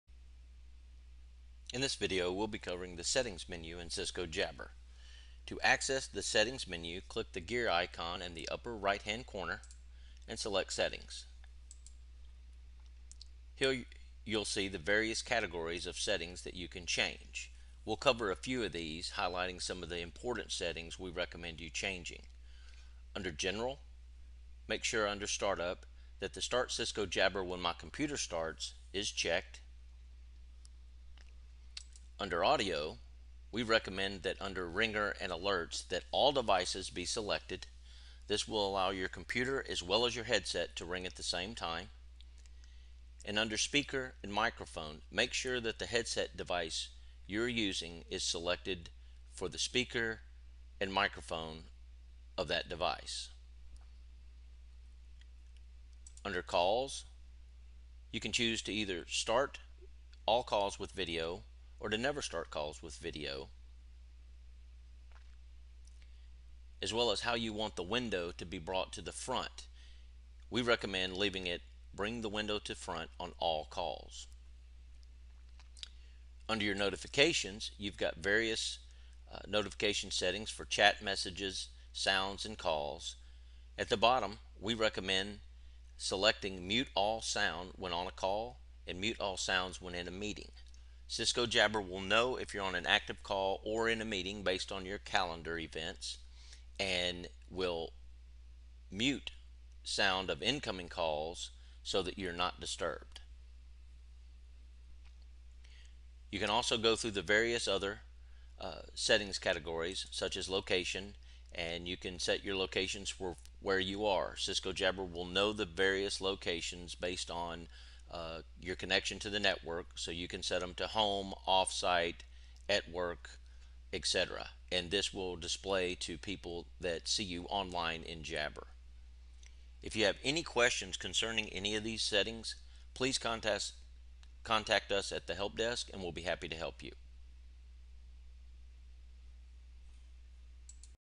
Cisco Jabber Settings Menu
Video covering the settings menu in Cisco Jabber and some of the most relevant settings.